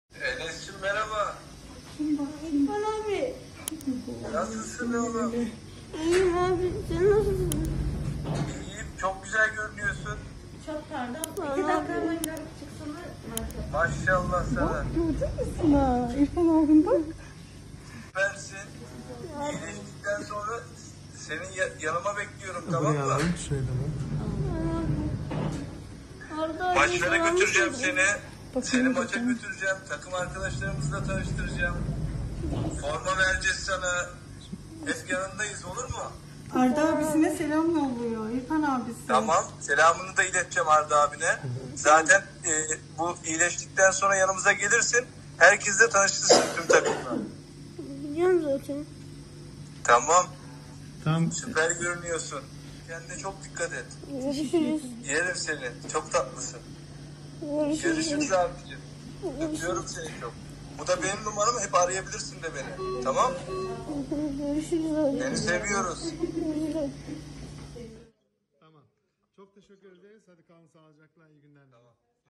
Konyalı doktor enkazdan çıkan çocuğu İrfan Can Kahveci ile buluşturdu
Kahramanmaraş merkezli 10 ili etkileyen depremlerde bir enkazdan sağ olarak kurtulan ve tedavisini Konyalı doktor Ahmet Nihat Baysal'ın yaptığı çocuğun isteği yerine getirildi. Depremzede çocuk Fenerbahçeli futbolcu İrfan Can Kahveci ile görüştürdü

📢 Güncel haberlerden anında haberdar olmak için;